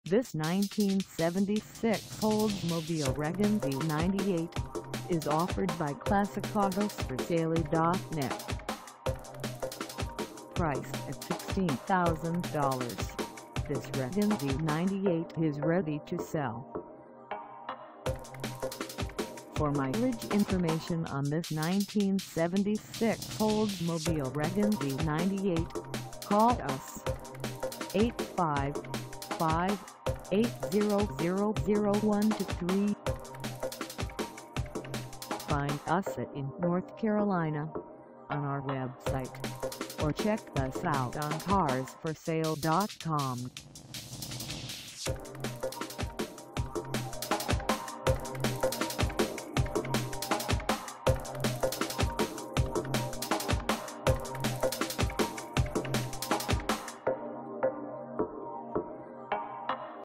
1976 Oldsmobile Regensy 98 - for sale in , NC 27603
Classicautosforsale.net

Come test drive this 1976 Oldsmobile Regensy 98  for sale in , NC.

Call For Location On This Fine Automobile  1976 original Olds Regency 98 in blue exterior and interior.Excellent condition.No rust whatsoever.1 owner.  We Offer Financing and Worldwide Shipping.  Call John to have your car listed on HaggleMe. It's Free!